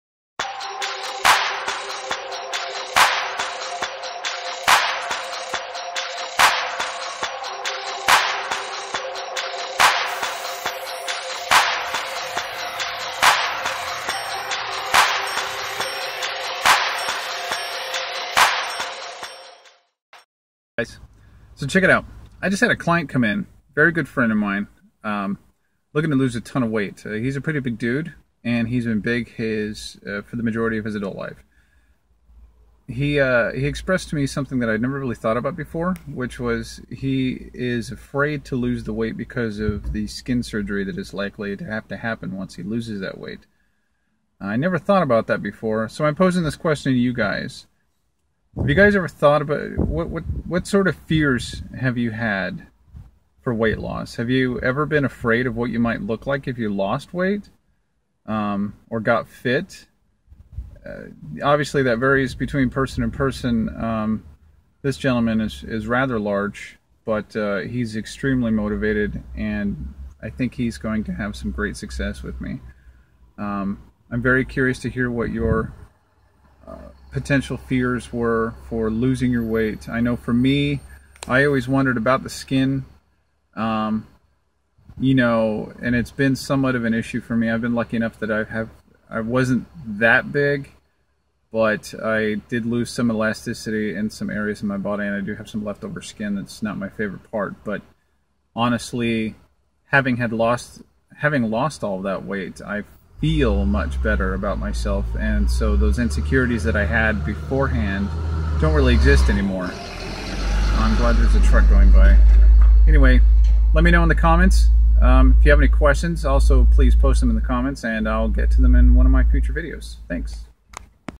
[Fit Philosophy] What scares you about getting fit?
It came to my attention today that some people fear what they might have to deal with if they lose a lot of weight.  What has or does scare you about transforming your body?